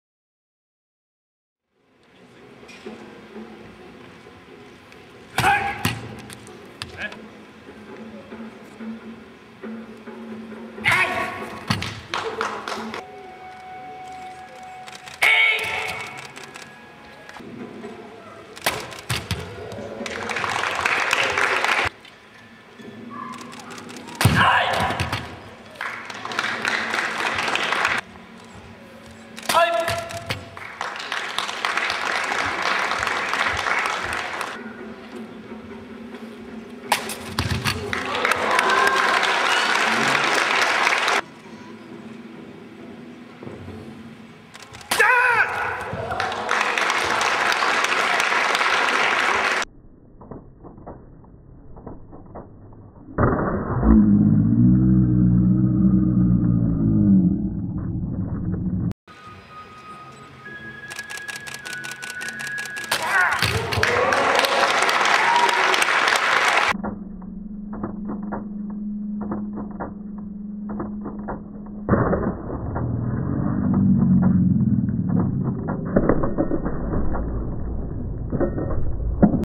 Dotan giri tameshigiri Porando Battodo Tai Kai 2009 | Samurai Juku
Dotan Giri competition (vertical cut through a stack of mats)  during Porando Battodo Tai Kai 2009 in Warsaw, Poland. All competitors in this division were members of Samurai Juku (Samurai School).